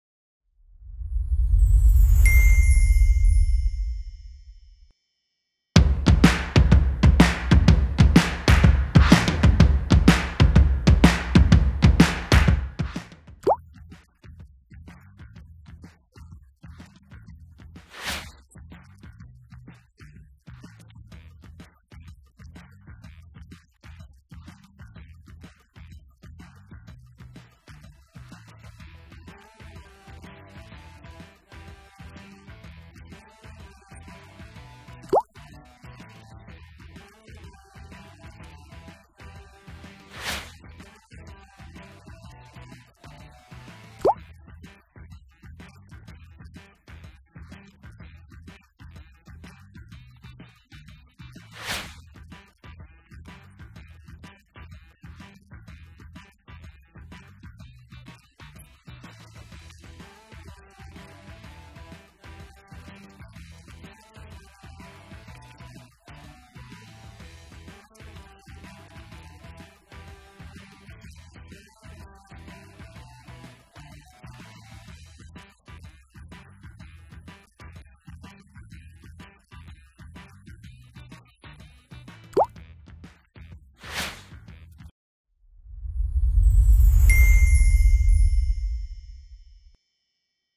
Kongsberg Ultimate_Print Report drupa 2024
Kongsberg Ultimate model received two prestigious professional awards in 2024. In an interview at Drupa 2024, Marko Pušec talks about the advantages of this model and the novelties it brings.